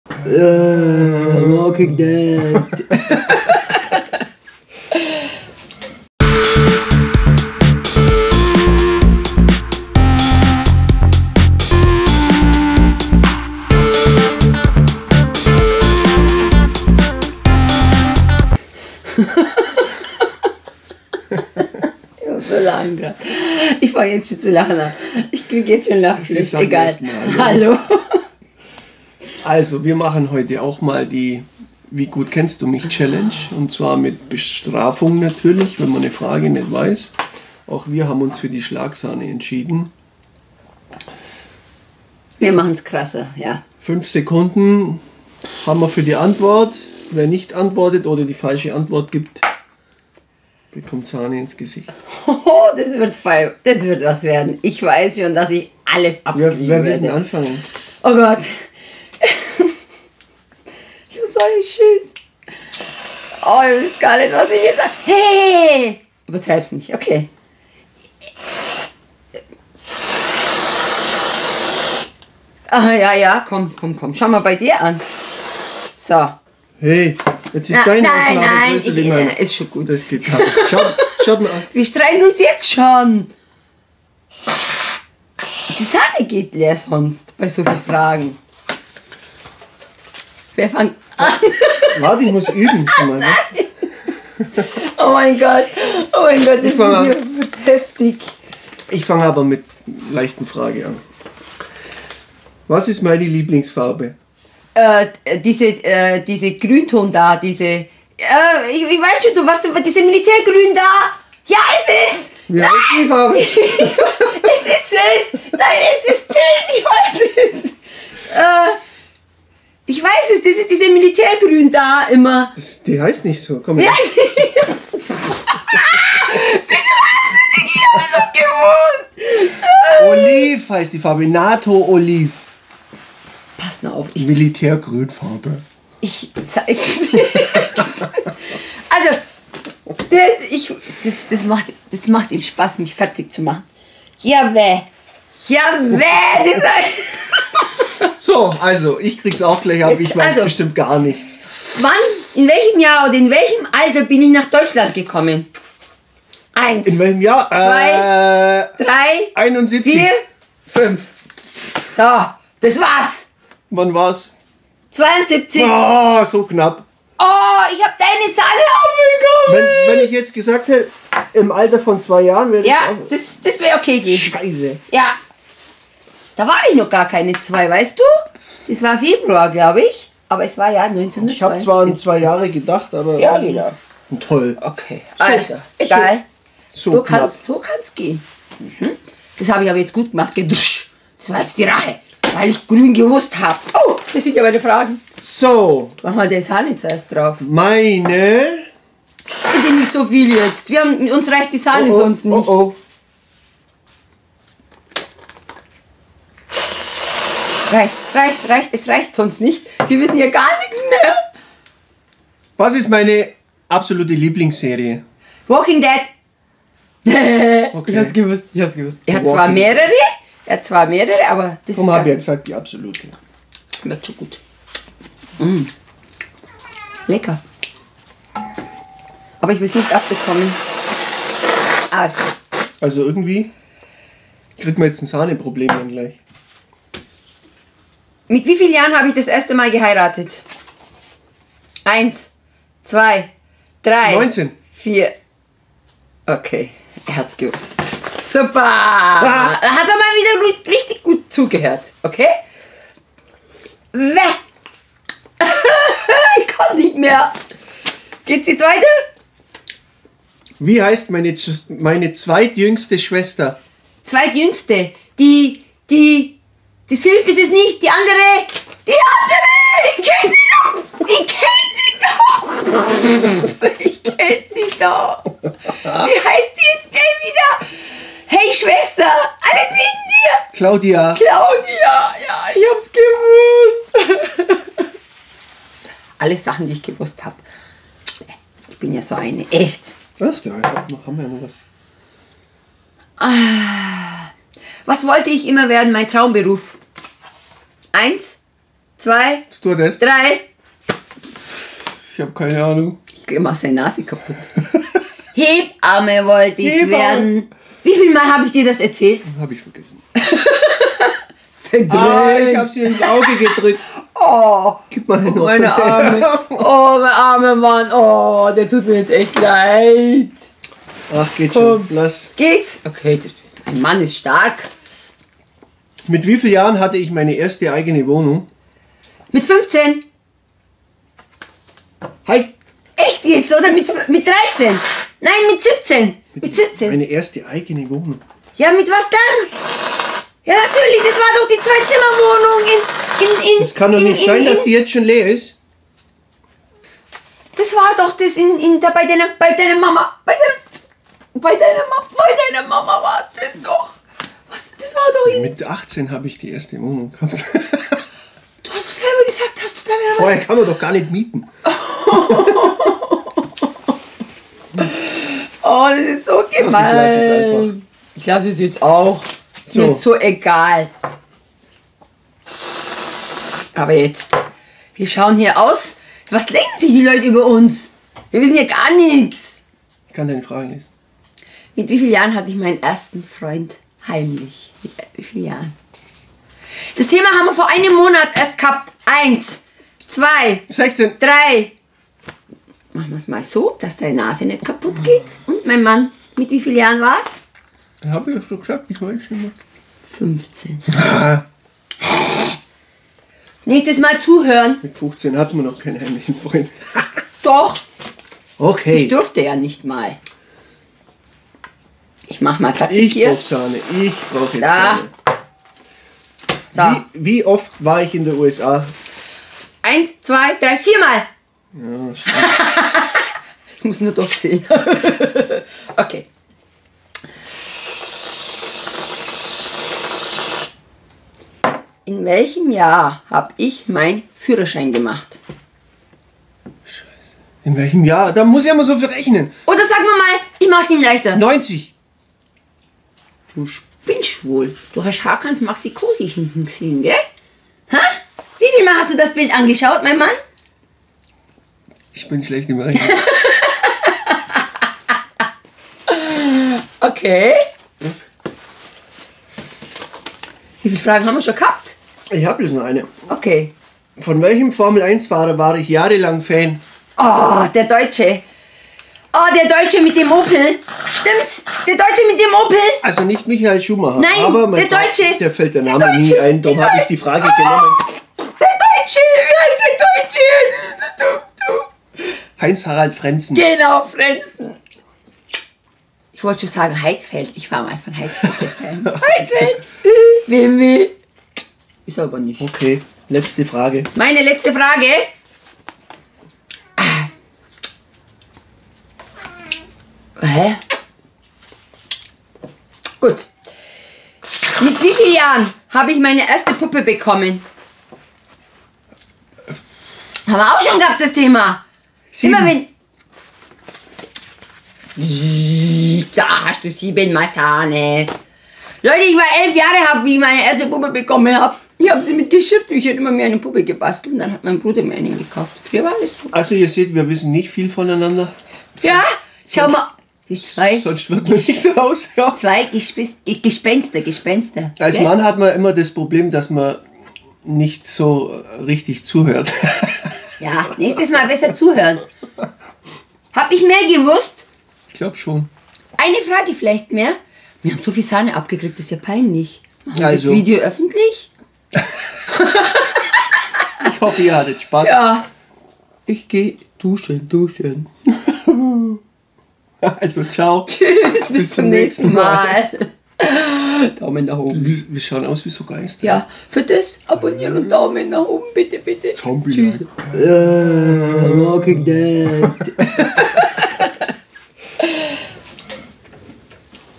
Wie gut kennst du mich ??? CHALLENGE mit Bestrafung | FAMILY FUN
Wie gut kennen wir uns? Wir stellen uns gegenseitig 7 persönliche Fragen. Wer die Antwort in 5 Sekunden nicht weiß wird sofort bestraft. Viel Spaß!

Liebe Grüße
Family Fun